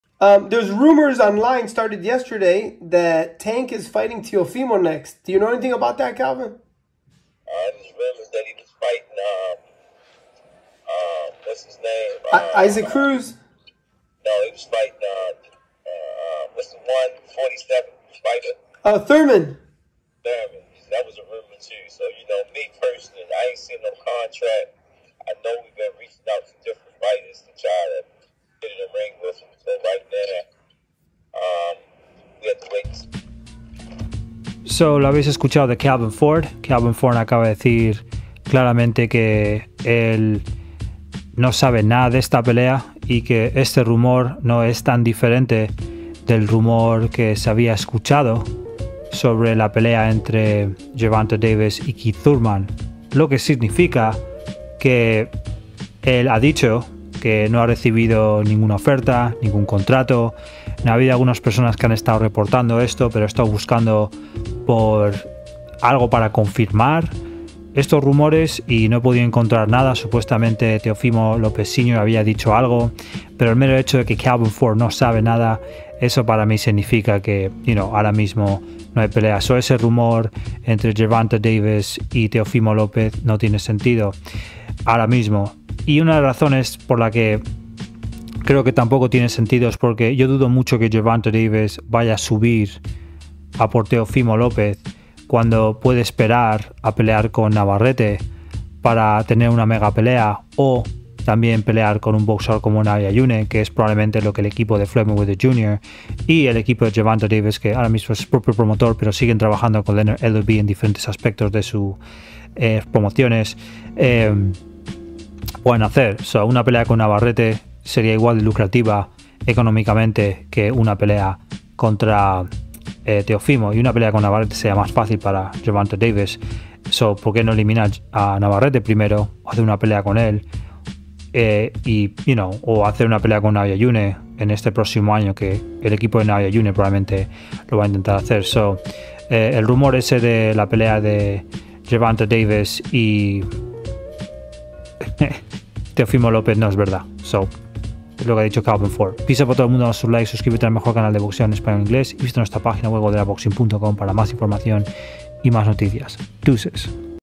Calvin Ford niega recibir una oferta de Teofimo Lopez para pelear contra Gervonta Davis
Calvin Ford habla sobre el rumor de la pelea entre Gervonta Davis y Calvin Ford